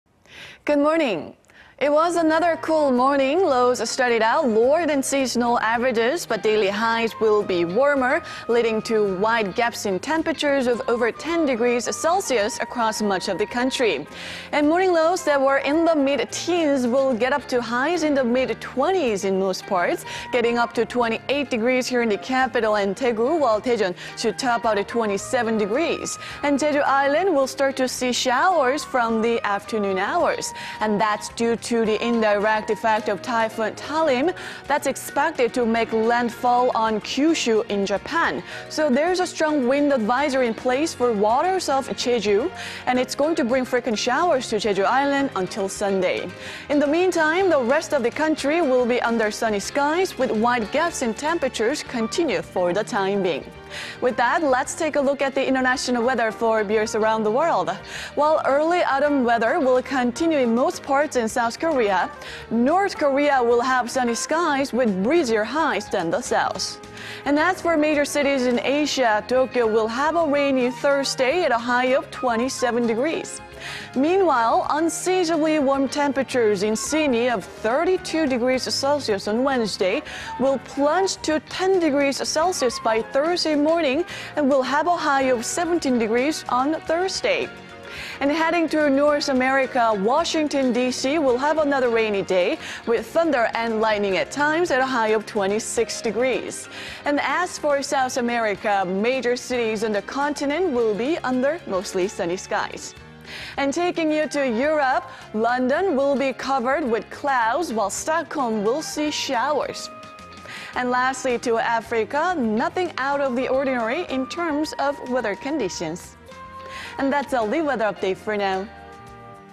Wide gaps in readings under sunny skies
큰 일교차, 맑은 하늘

  Good morning. It was another cool morning. Lows started out lower than seasonal averages, but daily highs will be warmer. Big gaps in temperatures of over 10 degrees Celsius will continue across much of the country.

  Morning lows that were in the mid teens will get up highs in the mid twenties...hitting to 28 degrees in the capital, Daejeon and Daegu,... while Gwangju will rise to 29 degrees. Jeju Island will see showers from the afternoon.

 That's due to the indirect effect of typhoon Talim, which is expected to make landflall on Kyushu, Japan. There's a strong wind advisory in place for waters off Jeju and it will bring frequent showers to Jeju Island until Sunday.

  Meanwhile, the rest of the country will be under sunny skies with wide gaps in the readings for the time being.

With that, let's take a look at the international weather for viewers around the world.

  While early autumn weather will continue in most parts in South Korea, North Korea will have sunny skies with breezier highs than the South.

As for major cities in Asia, Tokyo will have a rainy Thursday at a high of 27 degrees.

Meanwhile, unseasonably warm temperatures in Sydney of 32 degrees on Wednesday will plunge by 10 degrees by Thursday morning and will have a high of 17 degrees.

Heading to North America, Washington DC will have another rainy day with thunder and lightning at times.

As for South America, major cities on the continent will be under mostly sunny skies.

Taking you to Europe, London will be covered with clouds while Stockholm will see showers.

Lastly to Africa, nothing out of the ordinary in terms of weather conditions.